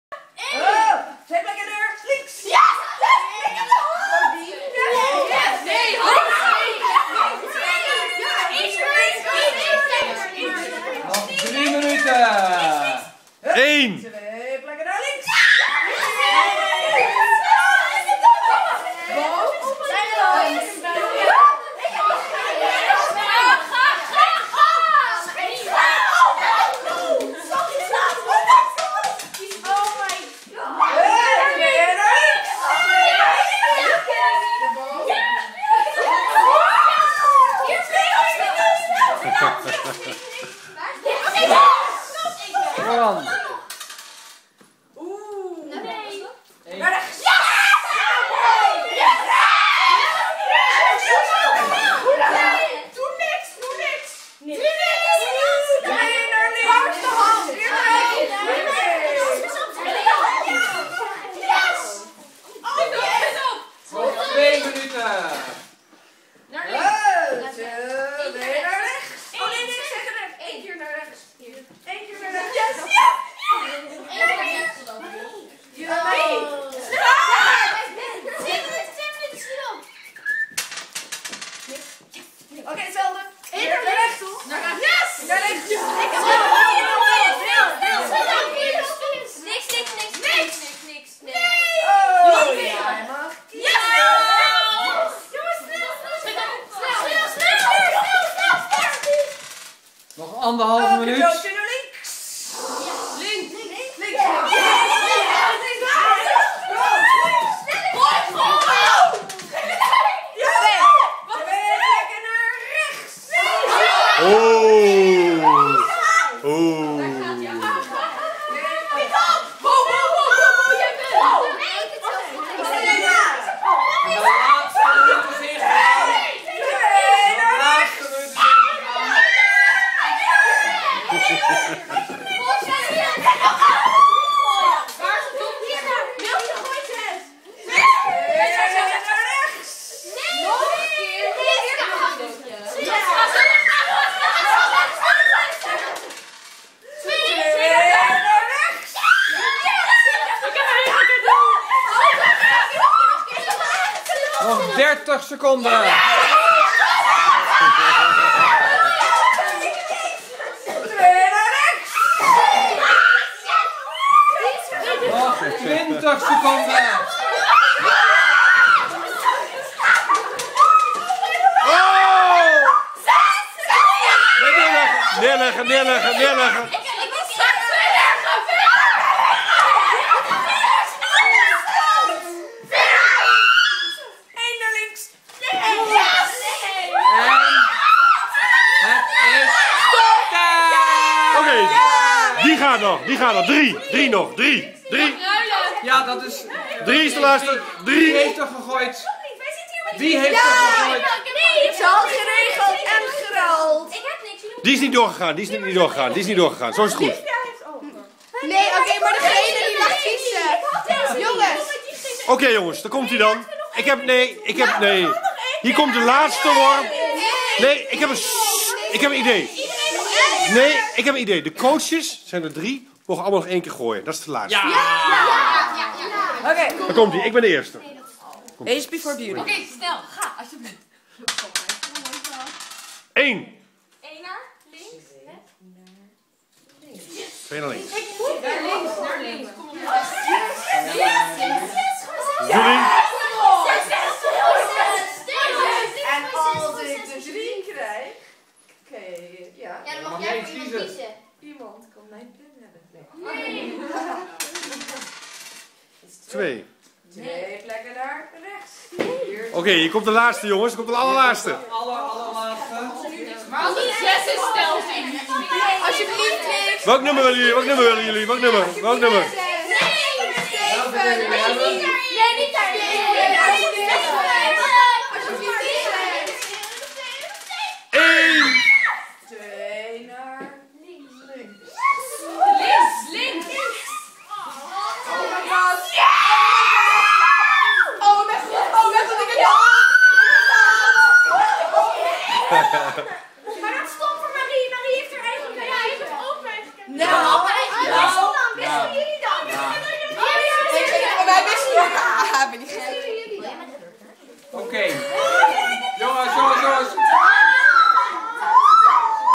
MC4 Sinterklaas 2011 part 1
Dobbelen om de kadootjes tijdens sinterklaas bij de Stoffertjes!